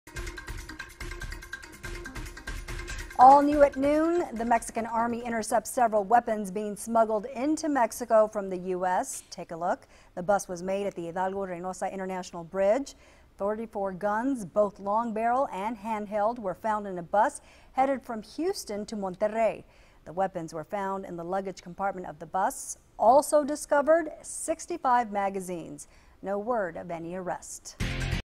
Mexican Authorities Seize Firearms at Hidalgo-Reynosa Bridge
A total of 34 guns were found in a bus headed from Houston to Monterrey.